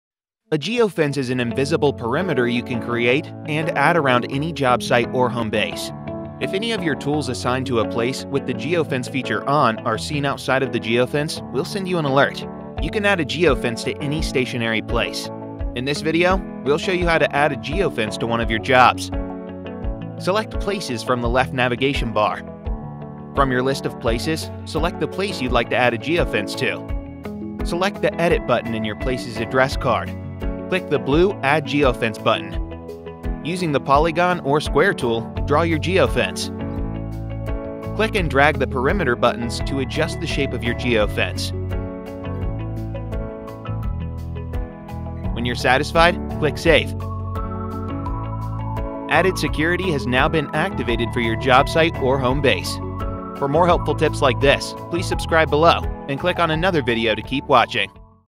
ONE-KEY™ Desktop: Add a Geofence
ONE-KEY™ features a free-to-use geofence tracker that lets you set an invisible perimeter around any jobsite and assign resources.  In this video, we'll show you how to add geofence tracking to one of your jobs using the One-Key web app. That way, you'll be alerted whenever any of your tools or equipment start to wander.